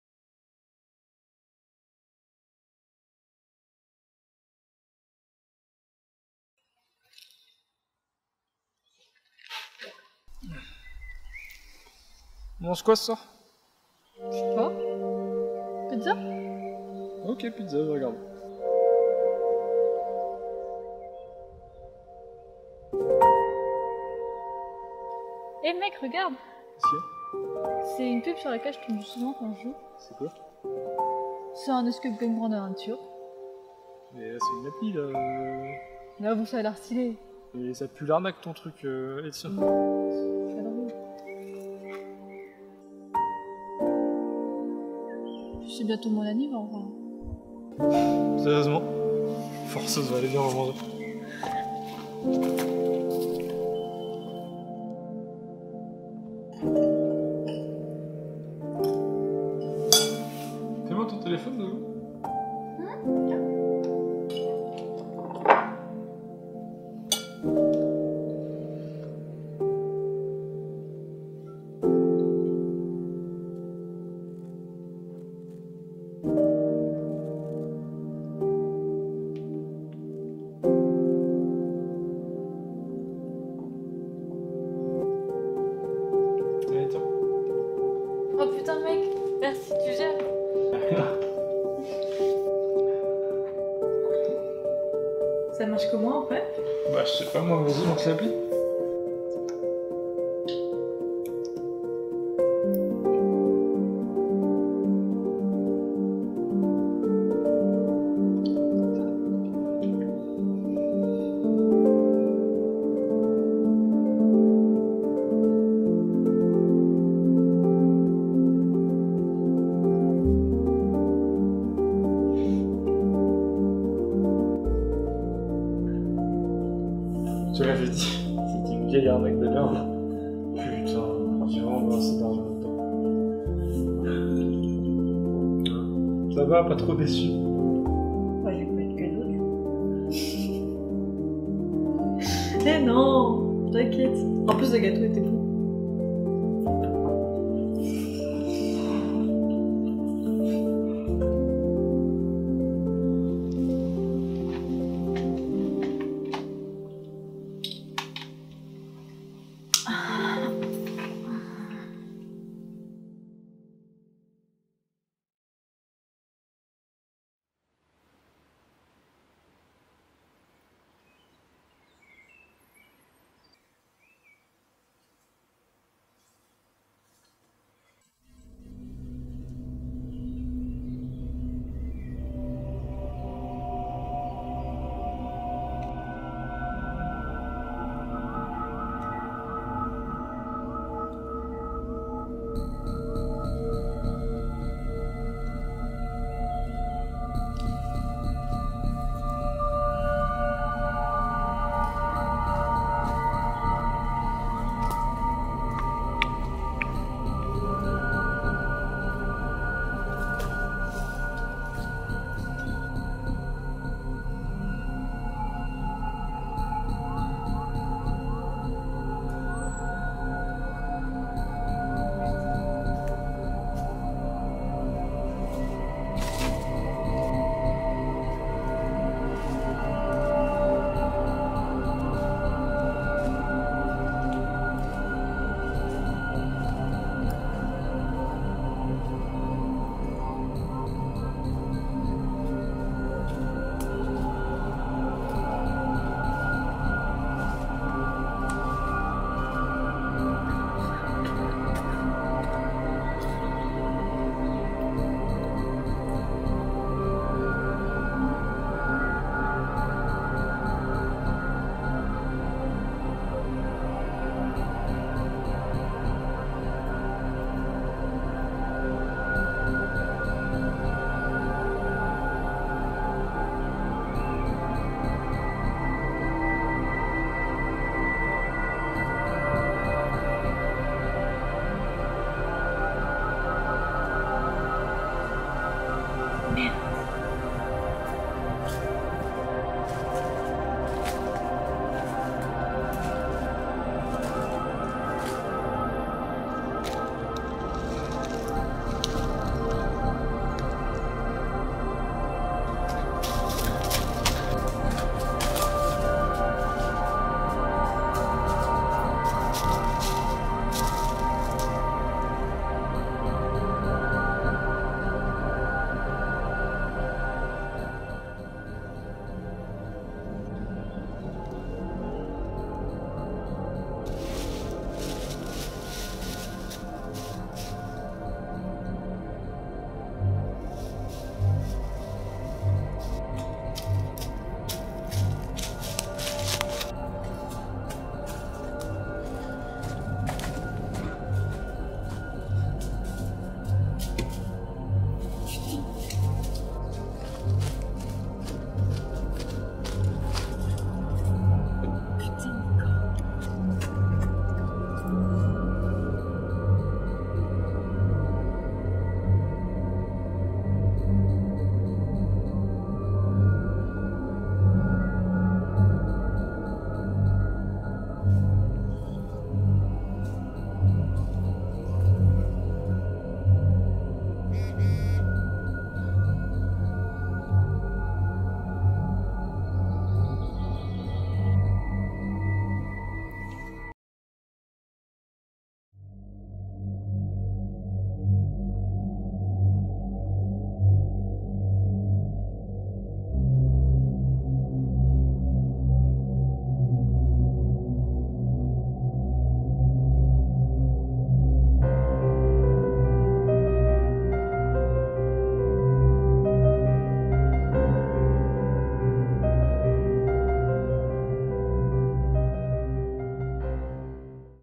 FF1J 2020 - MPS - Escape game
Film réalisé en 11j dans le cadre du Festival du Film d'un Jour
par l'équipe MPS
Thème 2020 "Béton"